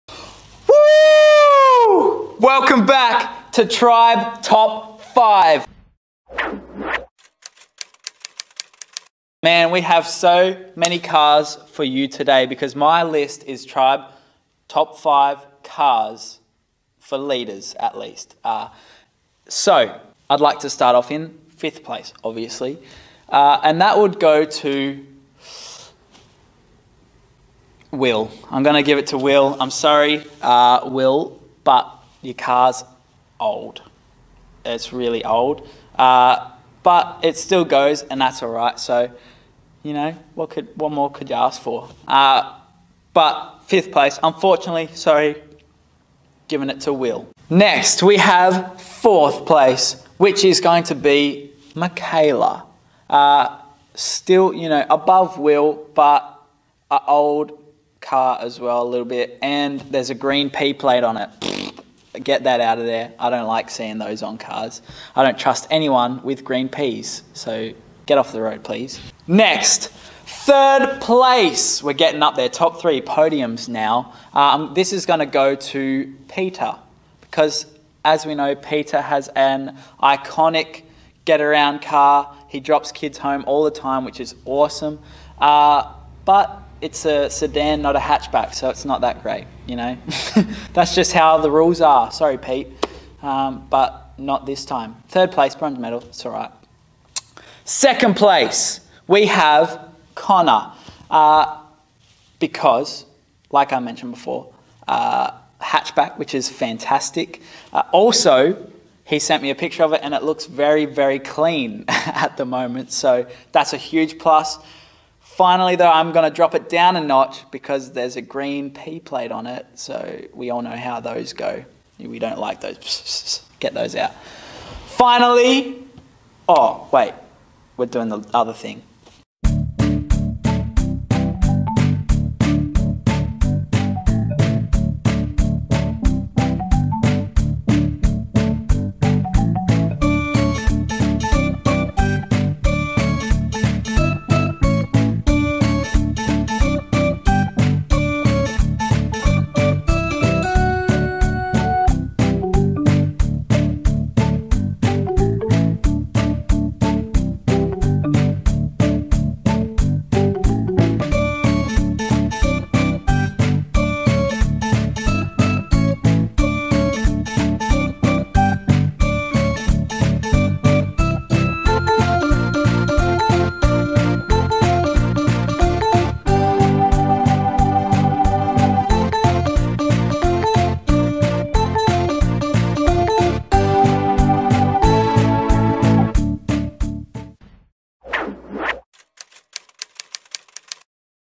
TRIBE TOP FIVE - YOUTH LEADERS CARS
TRIBE YOUTH FAMILY AND FRIENDS!
We are so excited for another episode if TRIBE TOP FIVE! This week we have Dylan with TRIBE YOUTH LEADERS CARS!

WHO HAS THE TOP CAR?
Dylan gave us a very detailed description as to why he thought that his Car should be Number One!

But we would like to know if you agree? Do you think it was a little biased and predictable? What about the Leaders Cars that weren’t involved?

We would love for you to comment and send us messages about what you think! What would your Top Leaders Car be?

We are missing you all so much, and we can’t wait to show you what else we have to come, so keep watching along!

We are hoping you are all enjoying being back at school, we would love to be there for you and encourage you in any way we can, so send us a message anytime!

Love from your,
TRIBE YOUTH LEADERS!